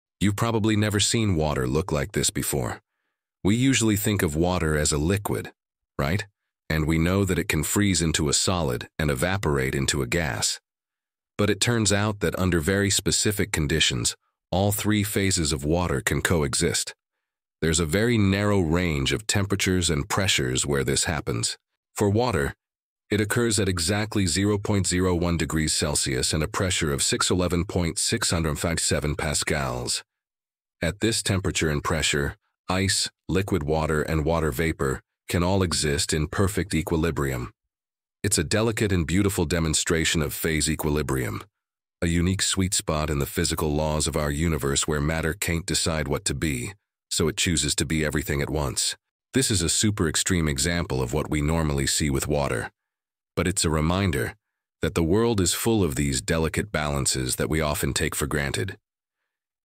Water Can Exist in Three States at Once
We’re taught that water can be a solid, liquid, or gas — but at one special point, it can be all three at the same time. Known as the “triple point,” this happens at exactly 0.01°C and 611.657 pascals of pressure. At this delicate balance, ice, liquid water, and water vapor coexist in perfect equilibrium. It’s a stunning showcase of nature’s physics, where water literally can’t decide what to be — so it becomes everything at once.